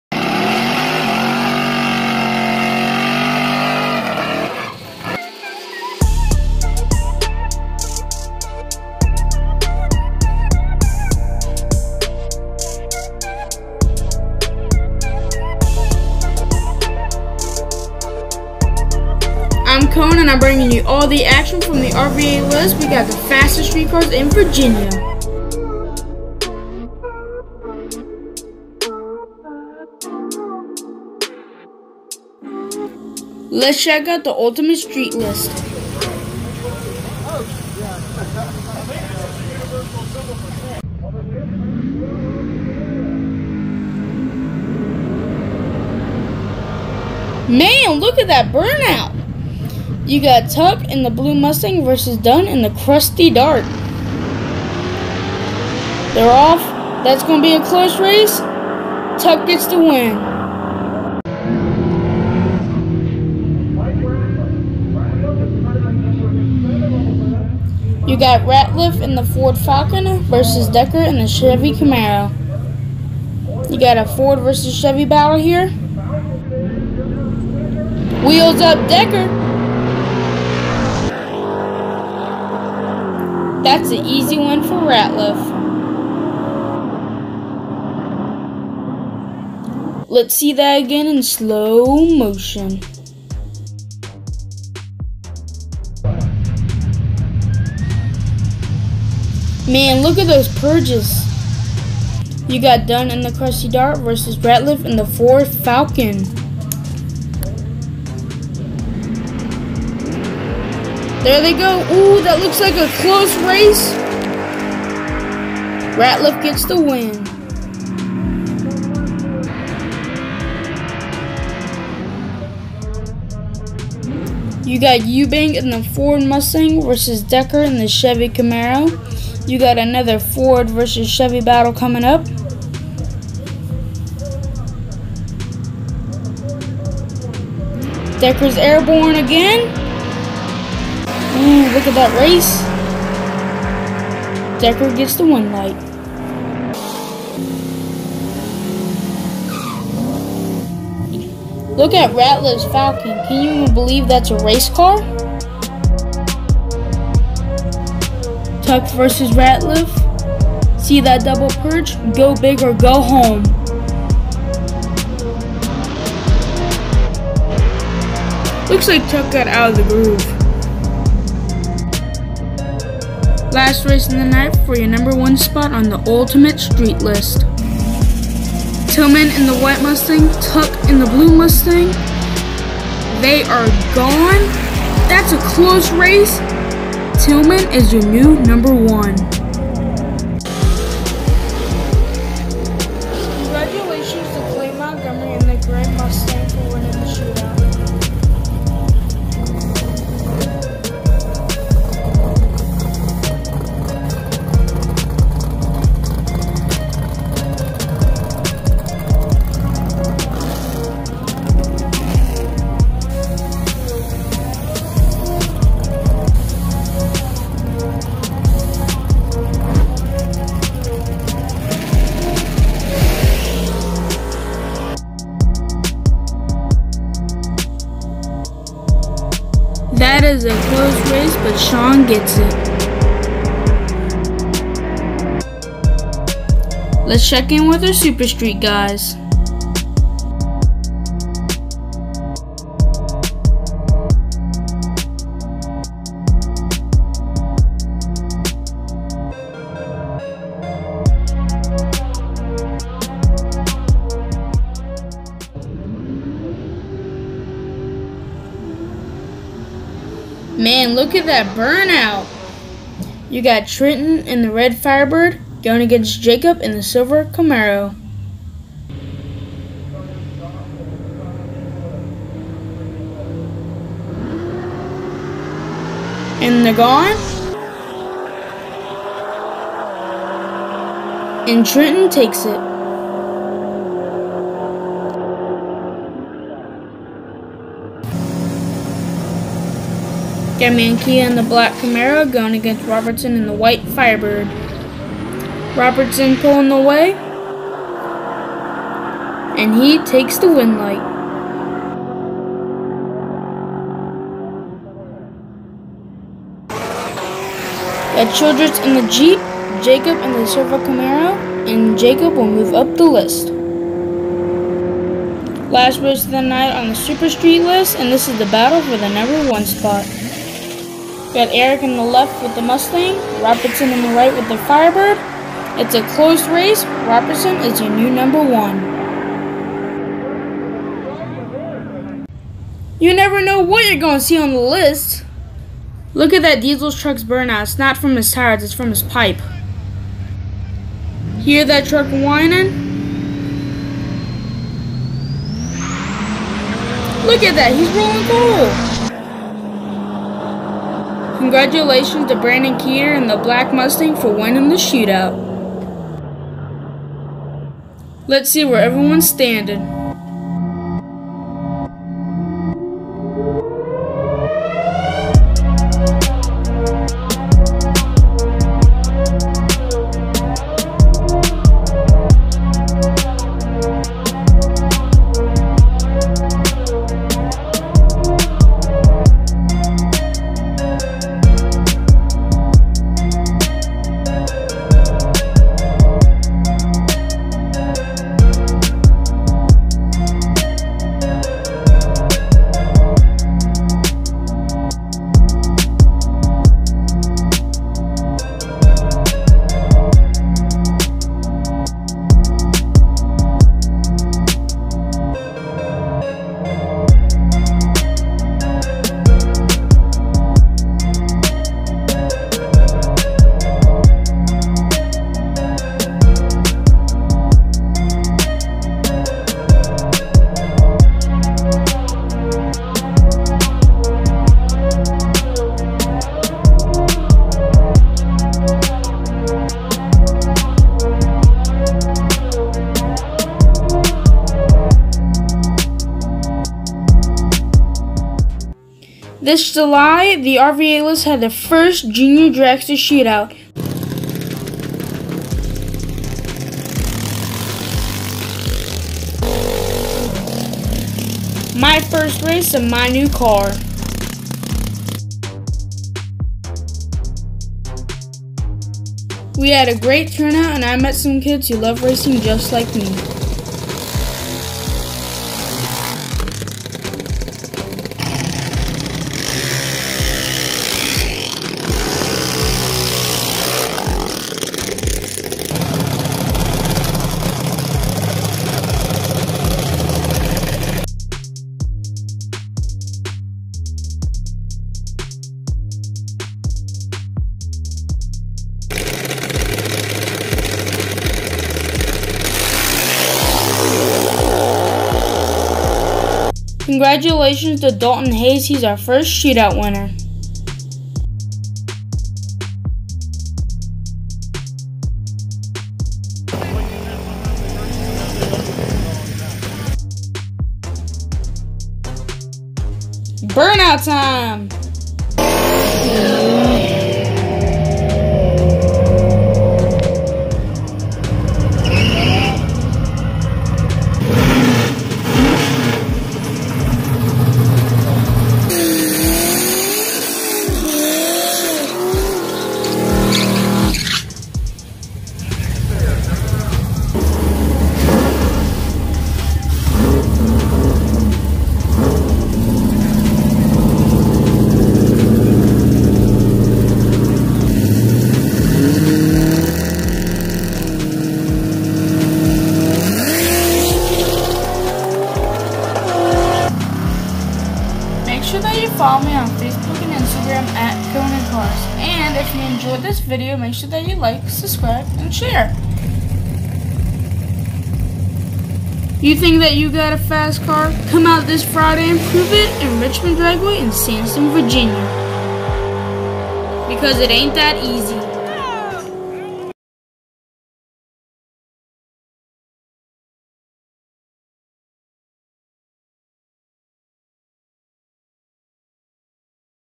RVA List July Recap
Decker does some wheelies, new # 1s, some sweet burnouts, and the first RVA List Jr Dragster Shootout.  Dont miss this extreme action at Richmond  Dragway and the fastest street cars in Virginia.

July 2019